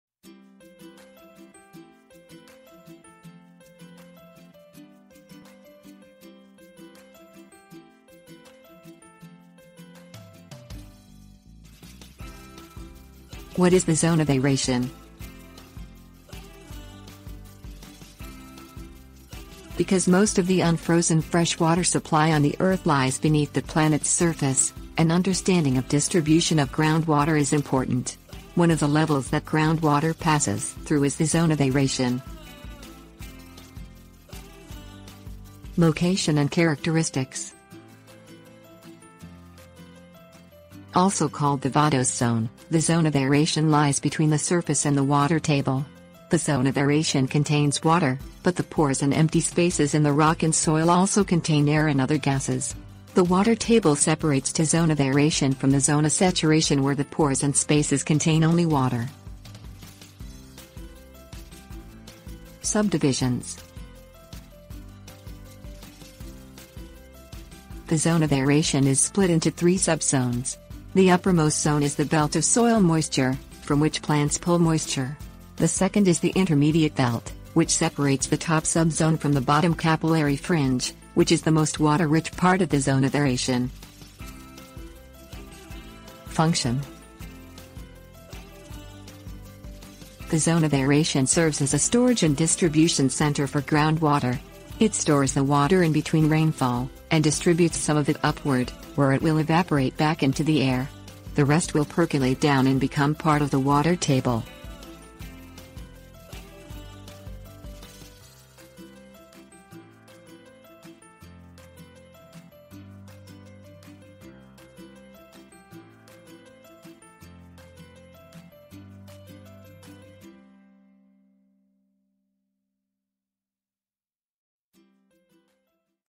What Is the Zone of Aeration?
What Is the Zone of Aeration?. Because most of the unfrozen fresh water supply on the earth lies beneath the planet's surface, an understanding of distribution of groundwater is important.  One of the levels that groundwater passes through is the zone of aeration.

Table of contents What Is the Zone of Aeration?
Location and Characteristics 00:34
Sub-Divisions 01:01
Function 01:25